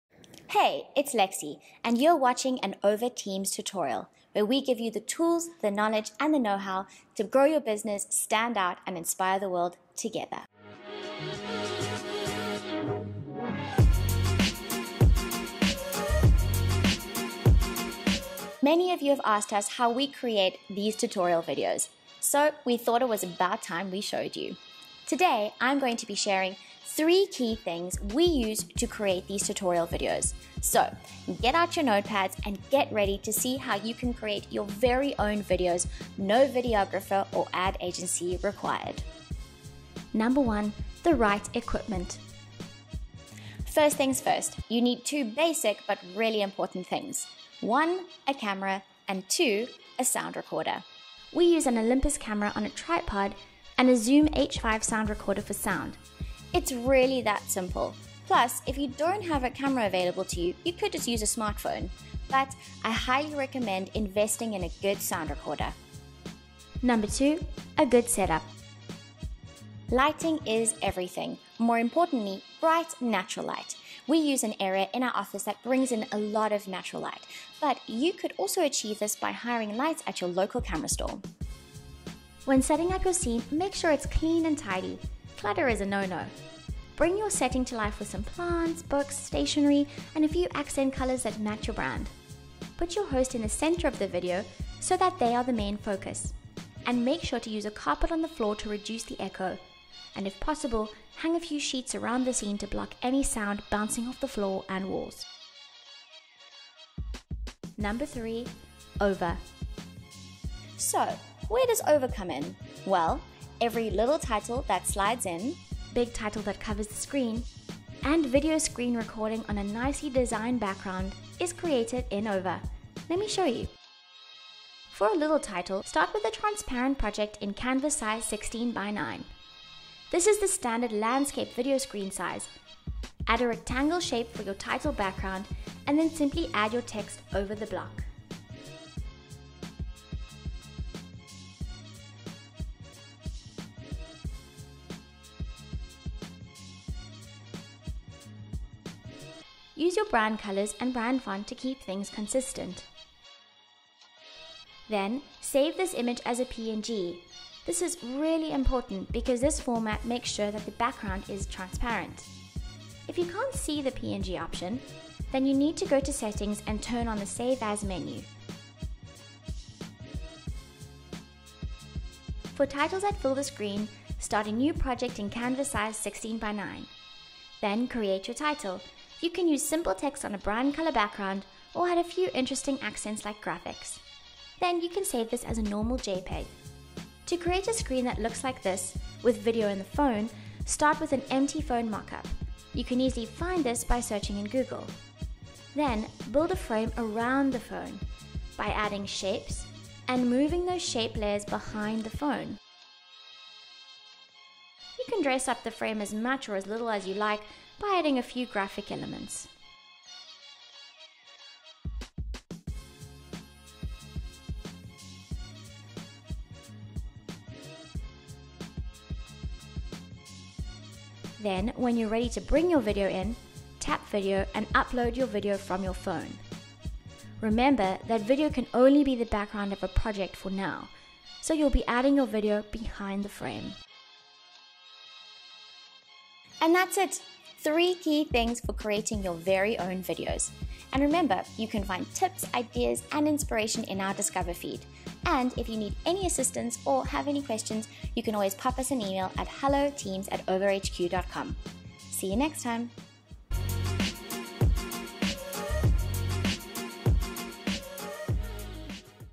How To Use Over to Create Your Own Tutorial Videos
Want you and your team to create your own unique tutorial videos, but don’t have the budget for a big production company? No worries, we’ve got you covered.

In this video, we’ll show you the 3 key things we use to create our tutorial videos and spoiler alert, one of them is Over.

You’ll learn:

- What equipment we use.
- How to record the sound.
- How to set up your space.
- How to create titles.
- How to create unique and stand-out screen recordings.

Watch now and start creating your own tutorial videos today!

P.S. Over Teams is only available in iOS.